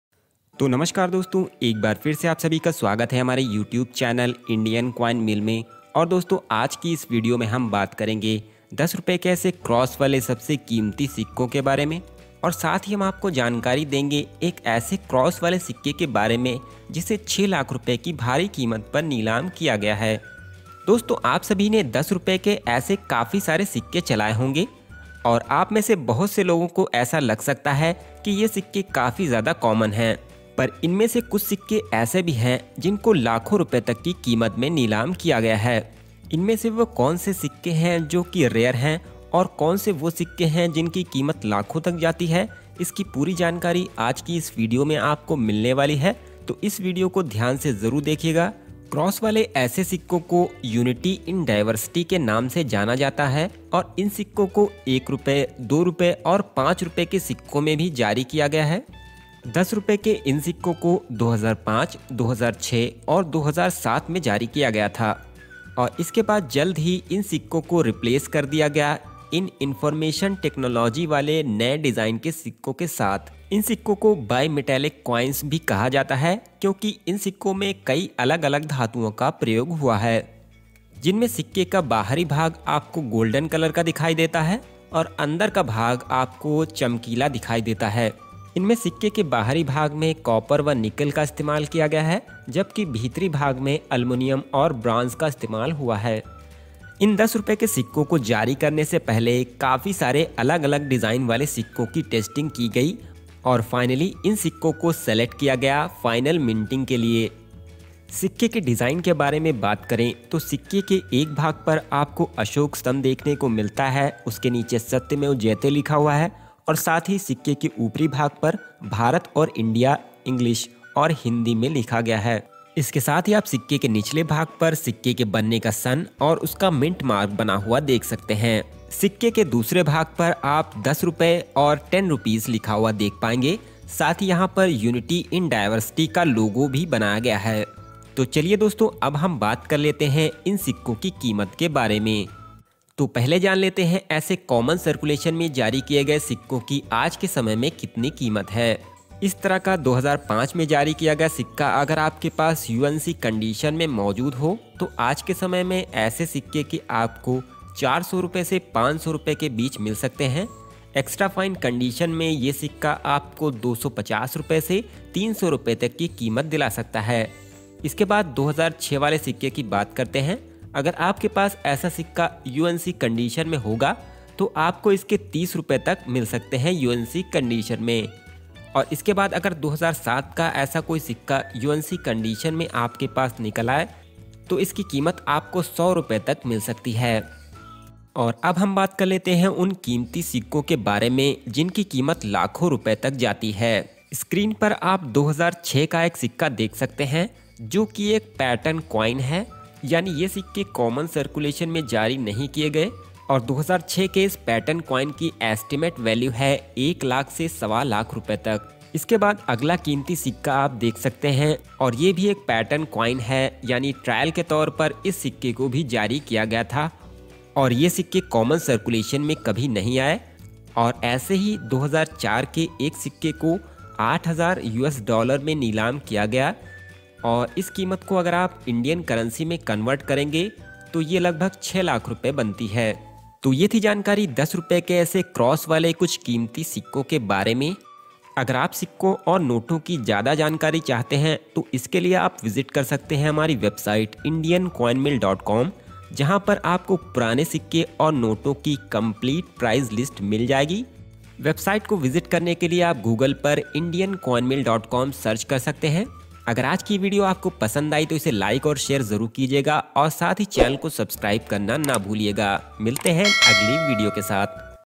10 Rupees Cross coin sold for ₹6 lakh | 10 रुपये का सिक्का क़ीमत 6 लाख | 10 Rs coin value
10 Rupees Cross coin sold for 6 lakh, a rare 10 Rs coin that is very difficult to find.
10 rupees cross coin is considered as a very common coin but some of them can give a good amount of value.
10 Rupee cross coins are also known as Unity in diversity coins which are also released in 1,2 and 5 Rupees coins also.